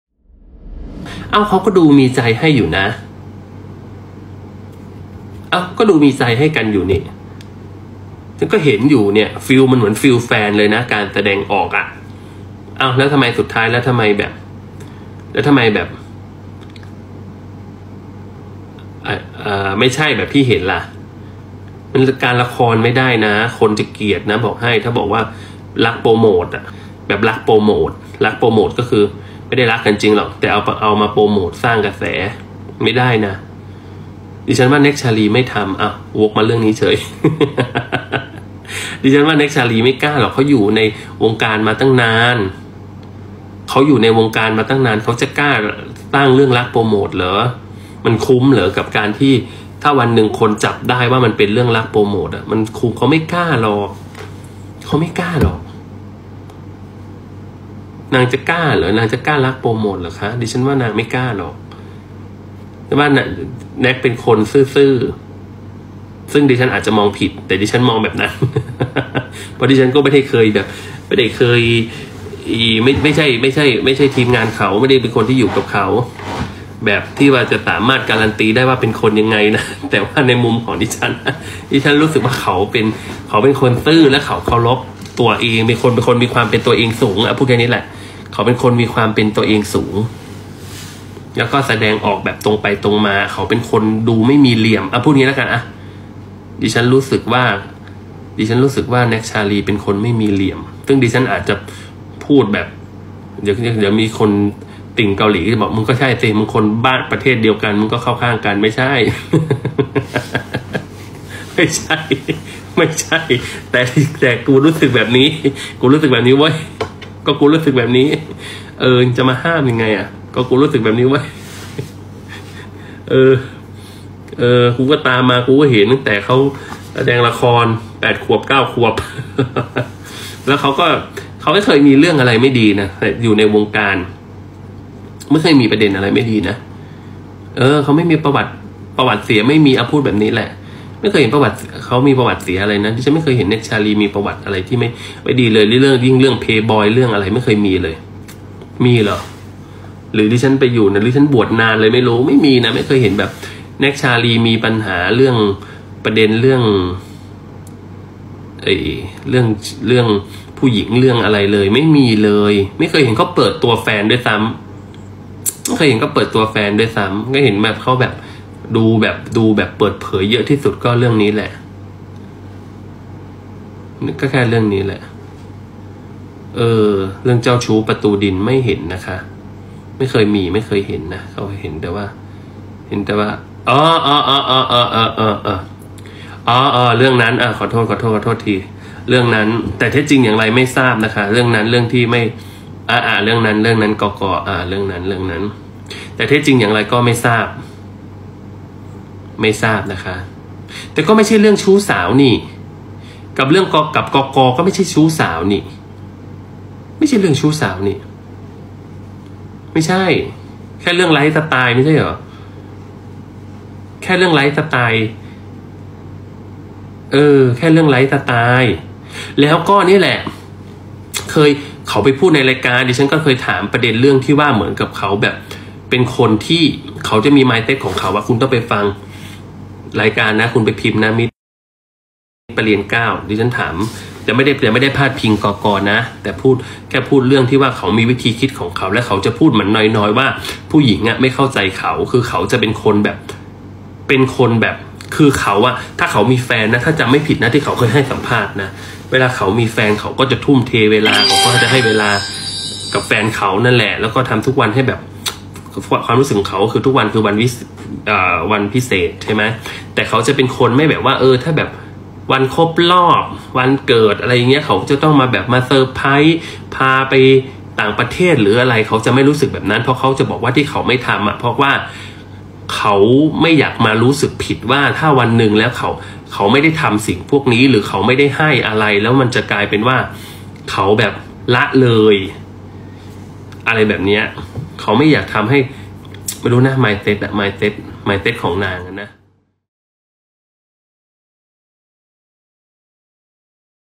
"แพรรี่" ลั่น การละครไม่ได้ คนจะเกลียด เชื่อ! "แน็ก ชาลี" ไม่มีเหลี่ยม ไม่หวังรักโปรโมต | Top Story
"แพรรี่" ลั่น การละครไม่ได้ คนจะเกลียด เชื่อ! "แน็ก ชาลี" ไม่มีเหลี่ยม ไม่หวังรักโปรโมต

📰 ติดตามข่าวสารเพิ่มเติมได้ที่